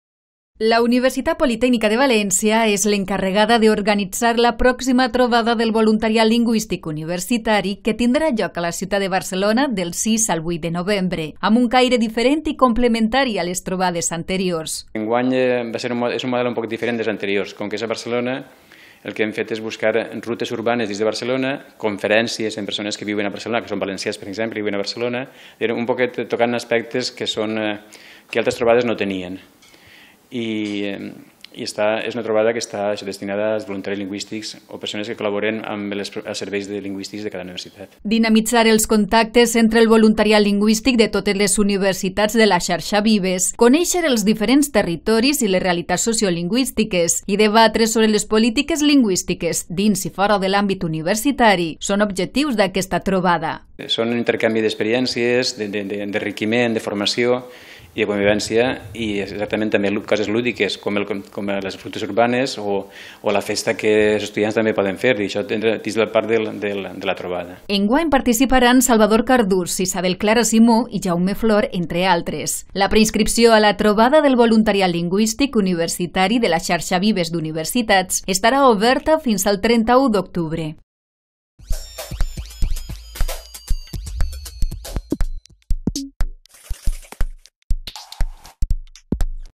Noticia @UPVTV: XV Trobada del Voluntariat Lingüístic Universitari [26-10-2015]
La Universitat Politècnica de València és l’encarregada d’organitzar la pròxima Trobada del Voluntariat Lingüístic Universitari, que tendrá lloc a la ciutat de Barcelona del 6 al 8 de novembre, amb un caire diferent i complementari a les trobades anteriors.

Dinamitzar els contactes entre el Voluntariat Lingüístic de totes les universitats de la Xarxa Vives, conèixer els diferents territoris i les realitats sociolingüístiques i debatre sobre les polítiques lingüístiques dins i fora de l’àmbit universitari són objectius d’aquesta trobada.

Enguany participaran Salvador Cardús, Isabel Clara Simó i Jaume Flor, entre altres.

La preinscripció a la Trobada del  Voluntariat Lingüístic Universitari de la Xarxa Vives d’Universitats estarà oberta fins al 31 d’octubre